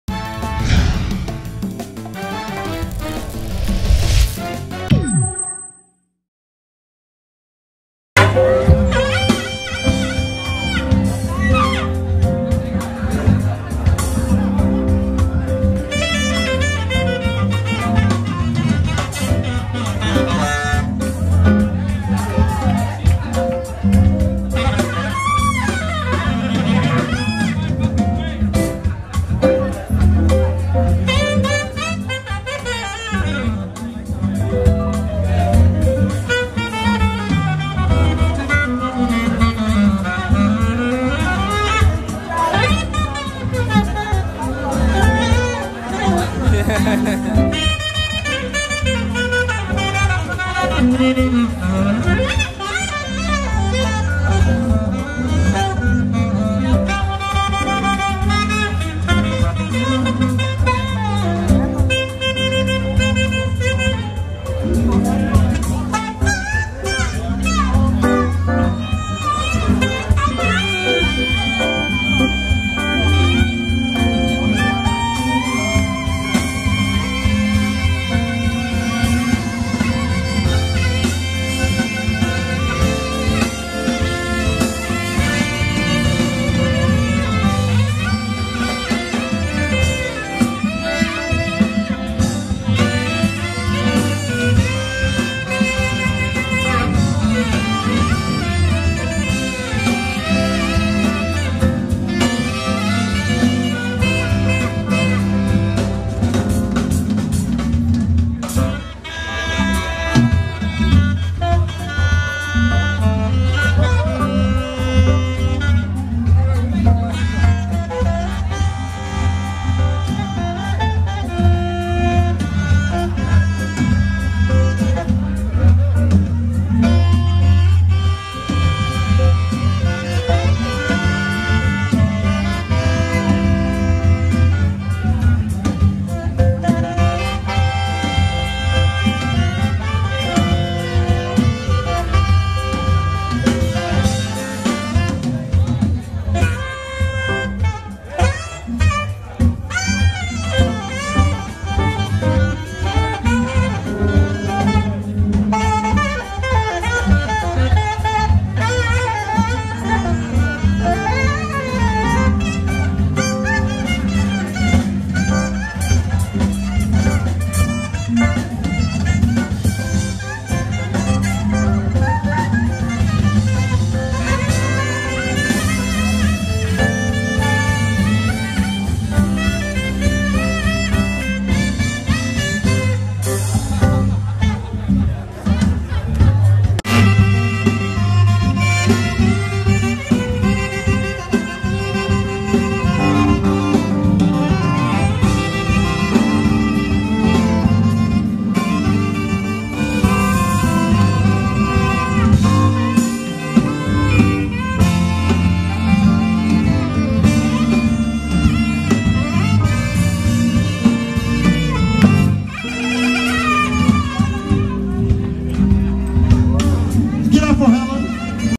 The Four Saxoteers | International Jazz Day 2013
Kenya's 'fourmost' jazz saxophonists, Don Williams Ouko, Joseph Hellon, Chris Bitok and Parseen Edwards jamming together during this year's International Jazz Day celebrations held at the Tree House Club, the first such celebration in Kenya